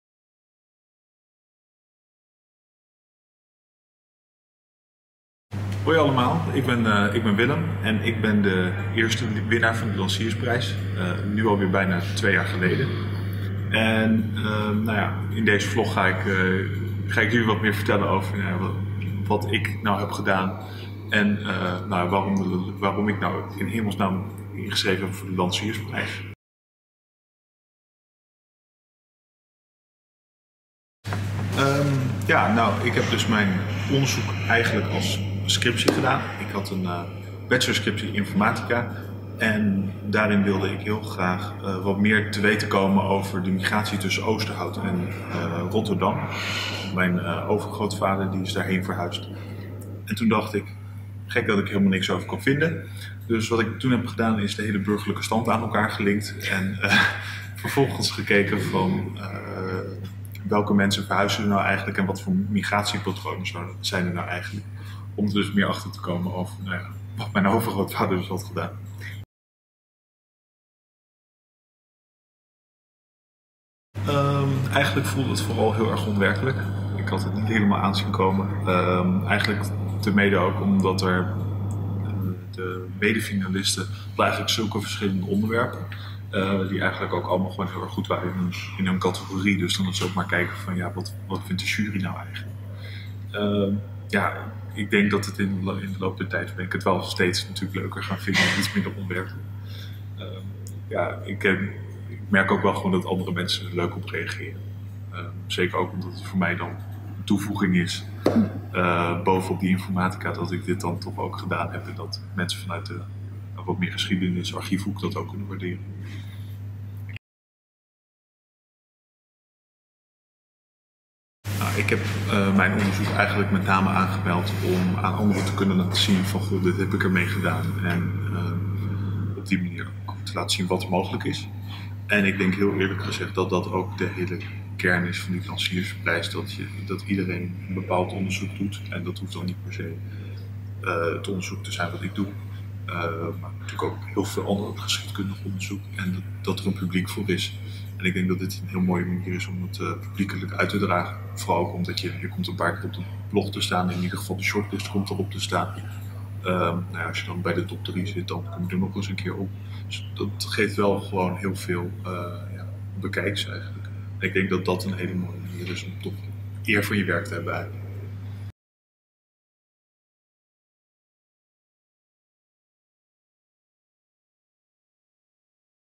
vlog #19 Winnaar Lanciersprijs 2017: Willem Vermeulen
Willem vertelt over zijn winnende inzending voor de Lanciersprijs 2017. Een aanmoediging voor onderzoekers om voor de nieuwe editie van 2019 hun onderzoeksresultaat in te dienen. Doe mee en maak kans op roem en een geldprijs.